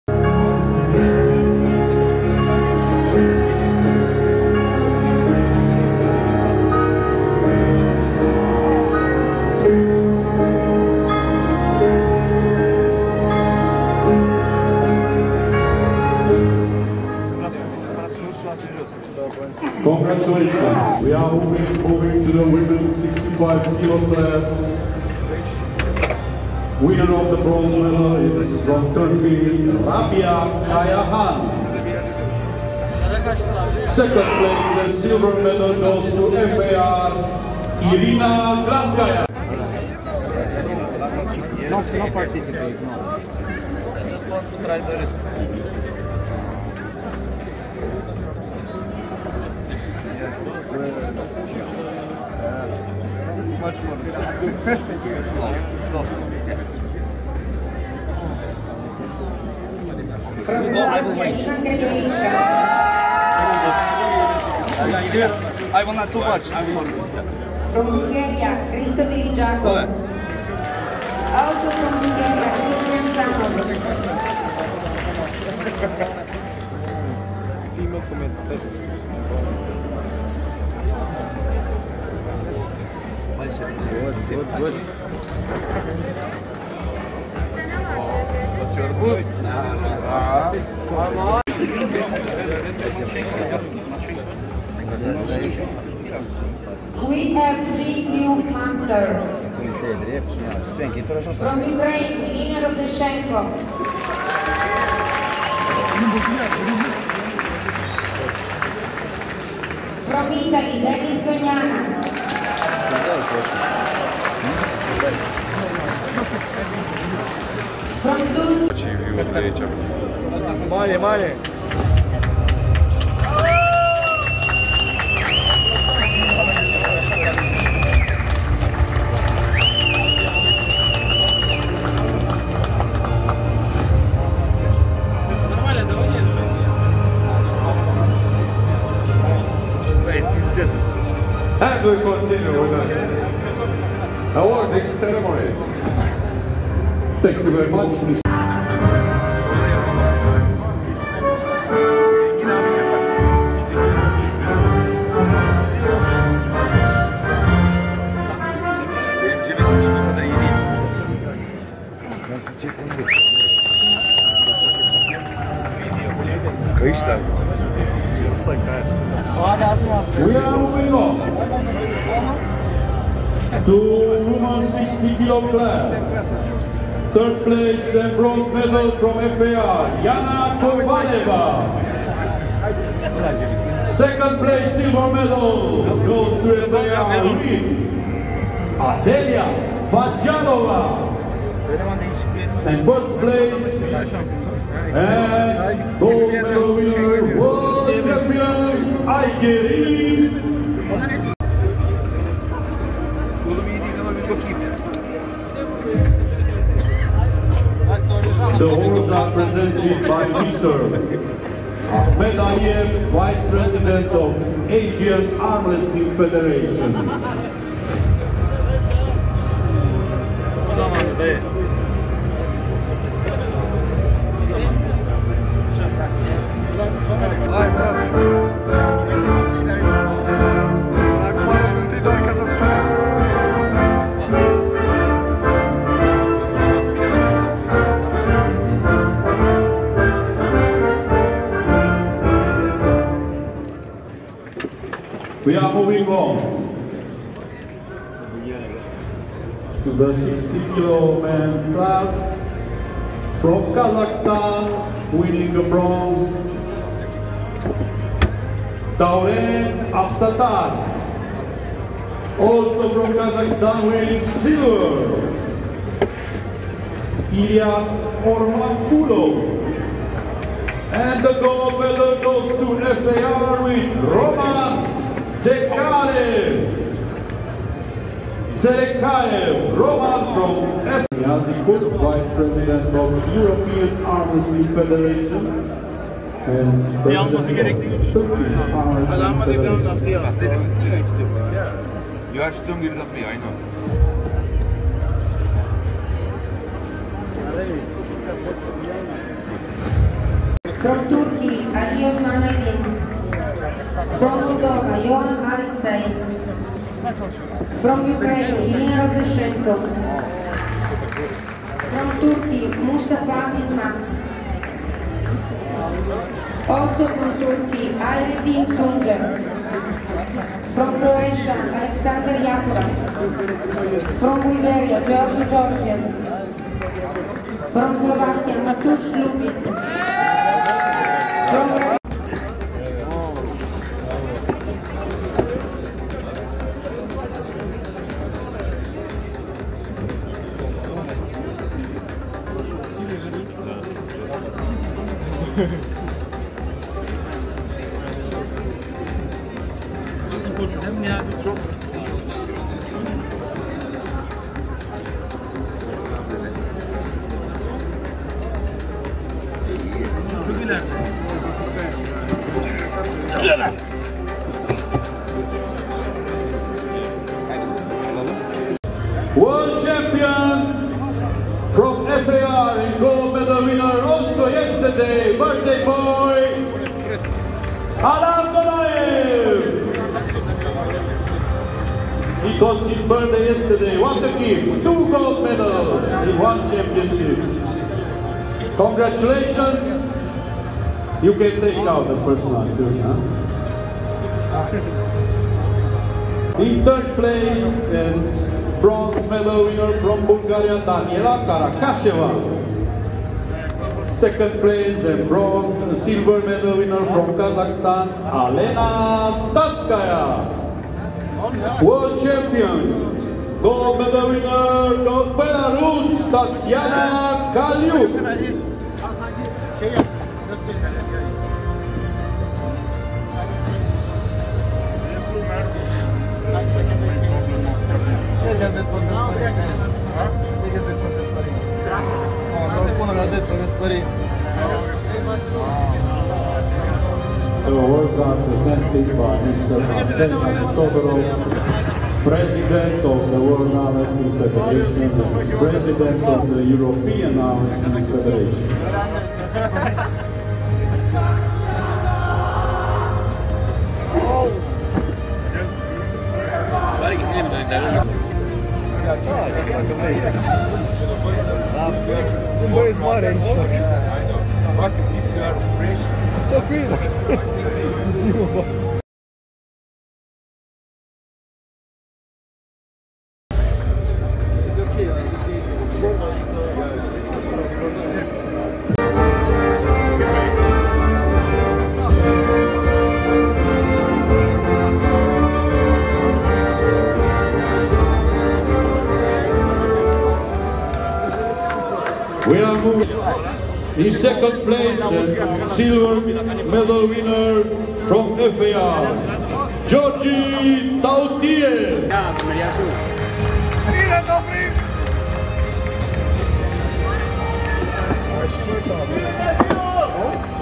WAC 2021 After Pulling (Alizhan Muratov, Engin Terzi, Bozhidar Simeonov Vaios, Aymeric, Jurovschi)
World Armwrestling Championship 2021 After Pulling with Alizhan Muratov, Engin Terzi, Vaios Anthoulis, Aymeric Pradines, Bozhidar Simeonov, Marius Jurovschi and many others.